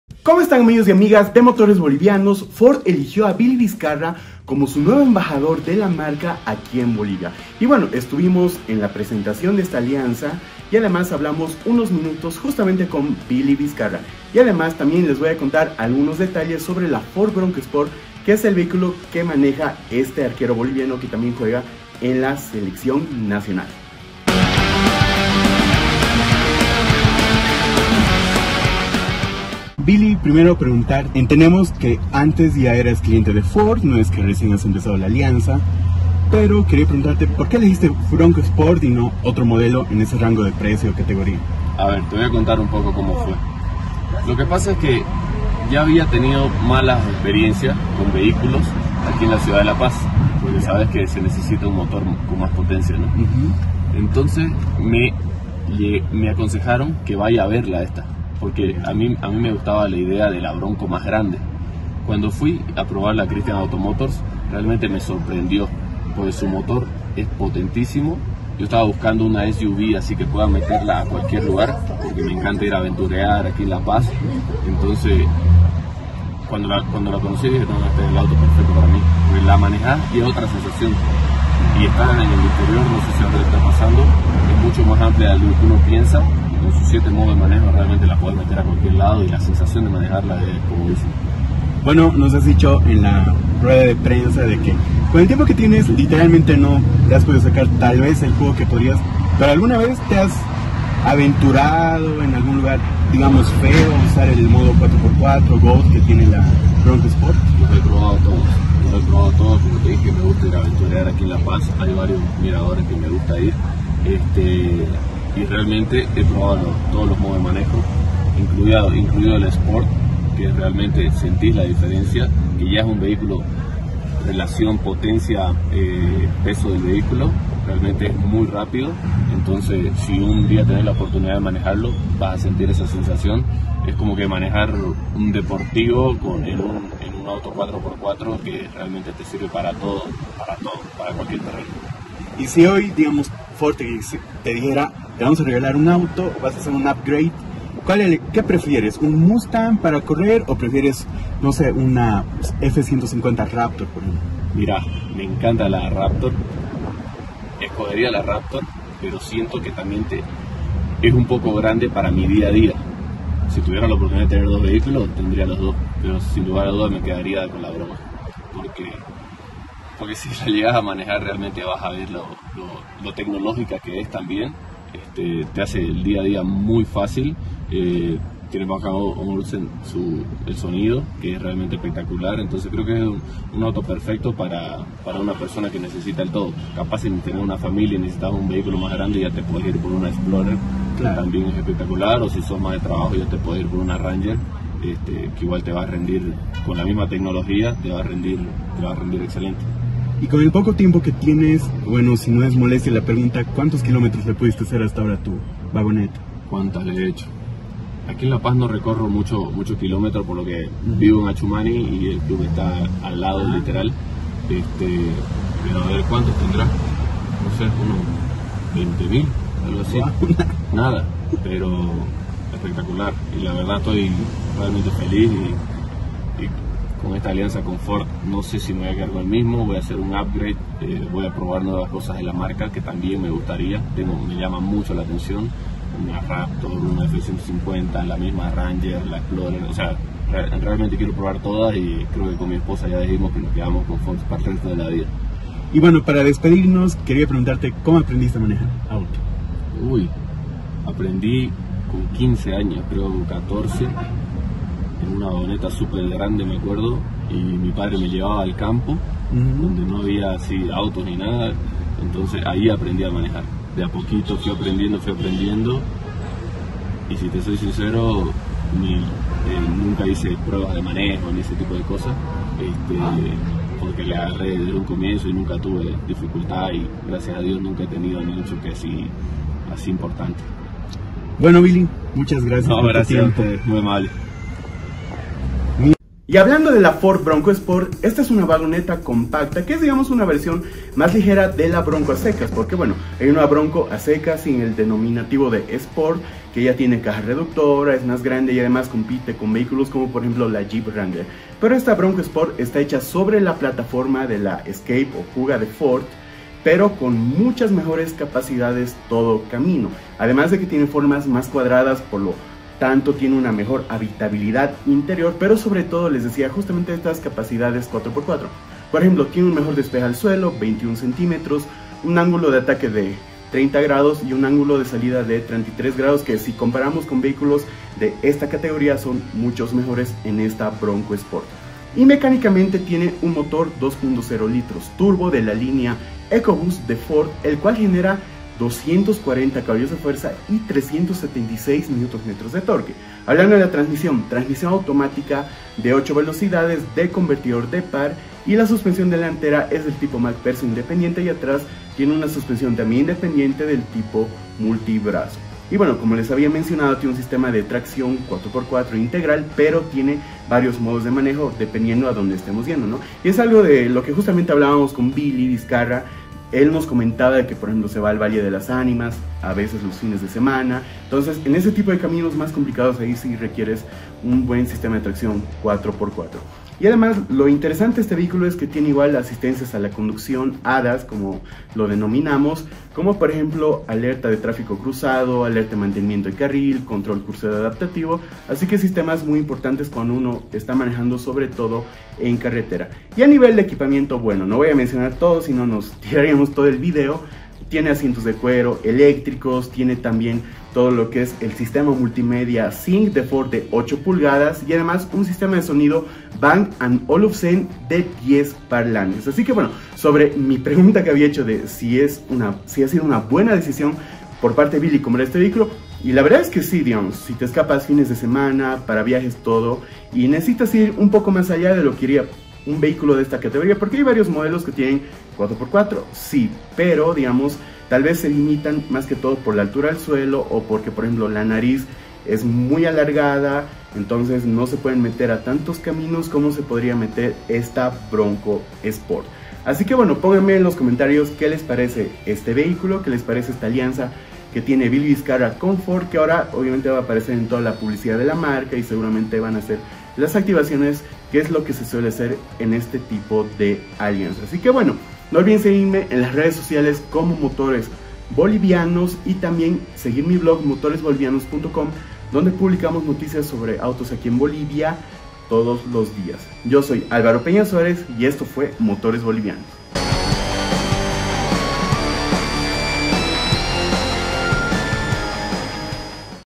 BILLY VISCARRA compra una FORD BRONCO SPORT, ¿eligió bien? (Nuevo embajador)│Motores Bolivianos
El arquero boliviano Guillermo "Billy" Viscarra fue elegido como embajador de Ford en Bolivia. ¿Por qué eligió este modelo? Él no los cuenta en este video.

¿Pensando en comprar un vehículo? 🧐
Asesorías de Motores Bolivianos 🚗
Conducción: Álvaro Peña Suárez│Instagram: @alvarin_mb
Cámaras: Gabby Valenzuela